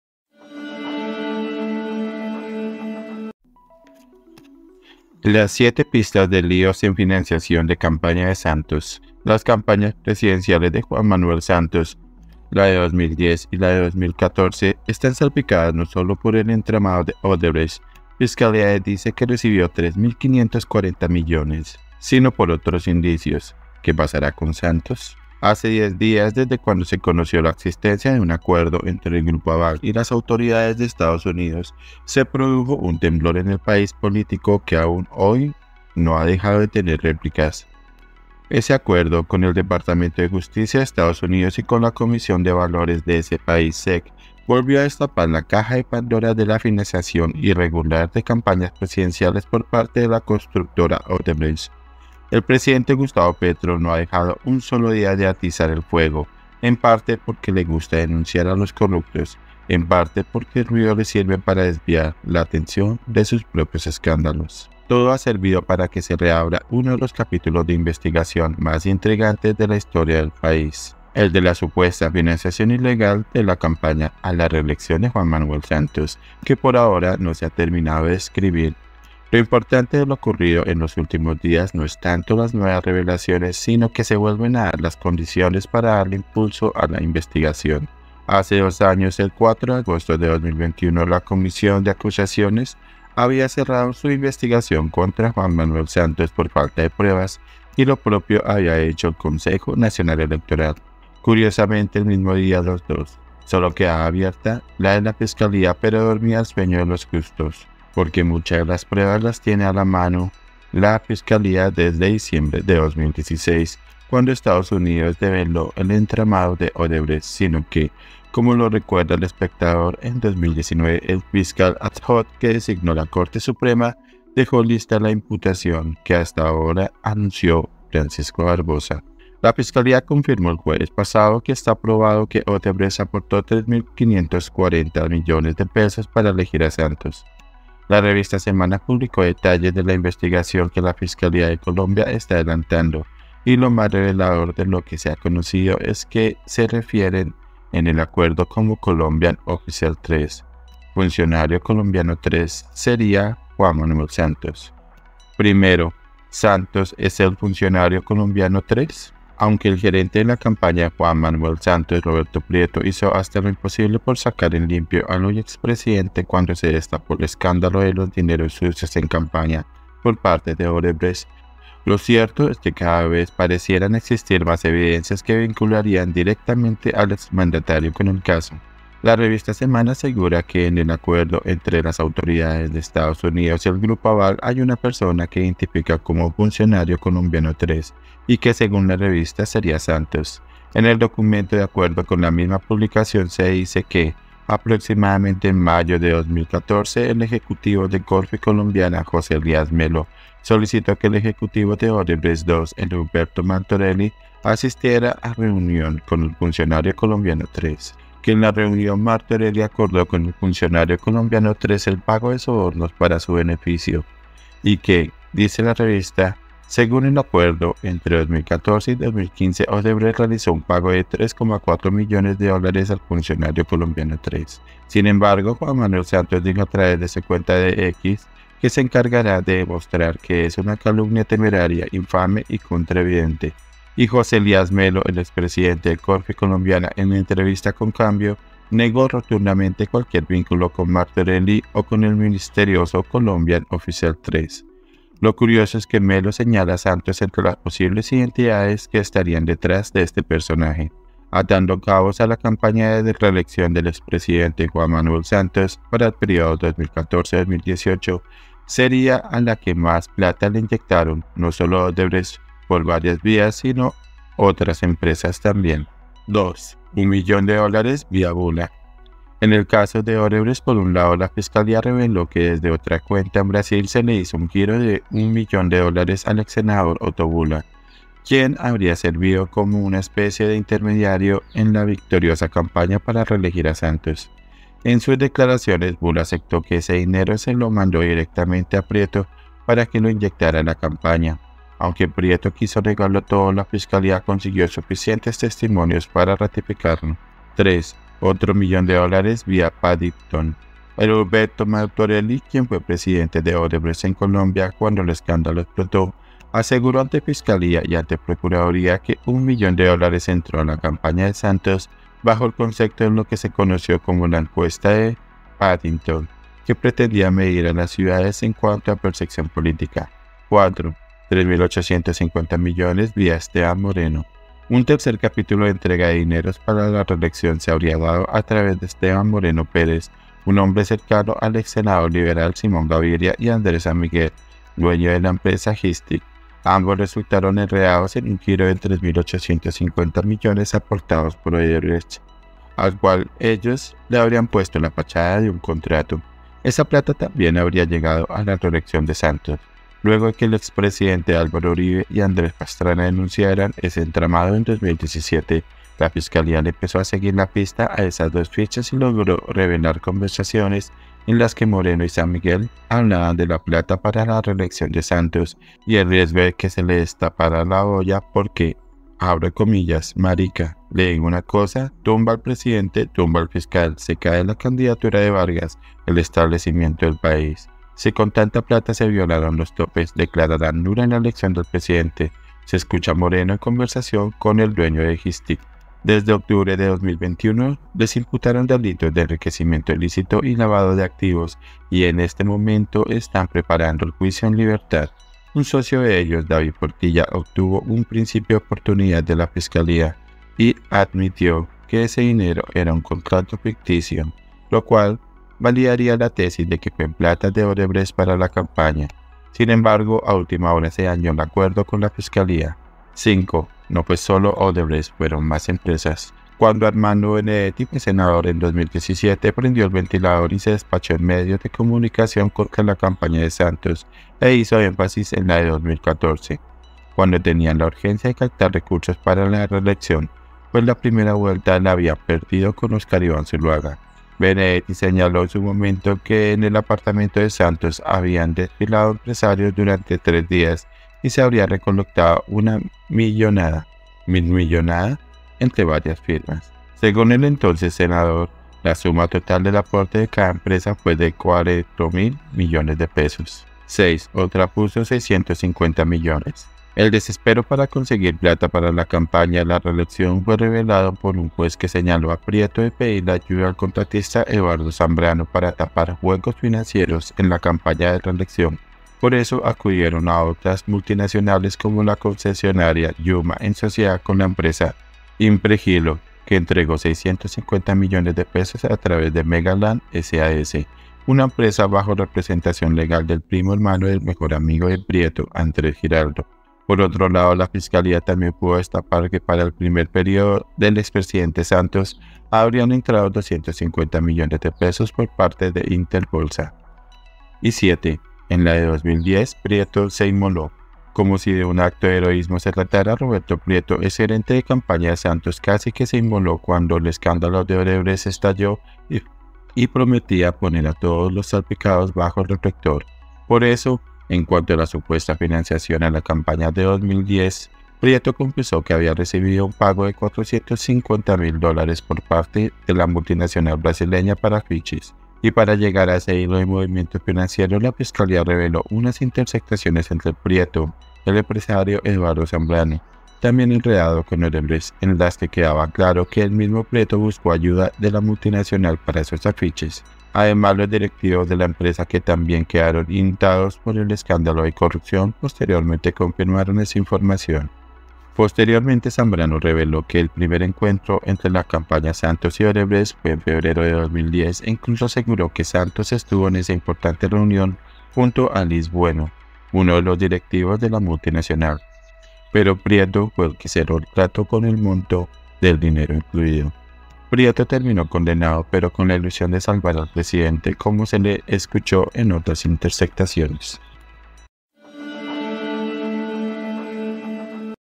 📢 LAS 7 PISTAS DE LÍOS EN FINANCIACIÓN DE CAMPAÑA SANTOS
Las campañas presidenciales de Juan Manuel Santos, la de 2010 y la de 2014, están salpicadas no solo por el entramado de Odebrecht –Fiscalía dice que recibió $3.540 millones– si no por otros indicios. ¿Qué pasará con Santos?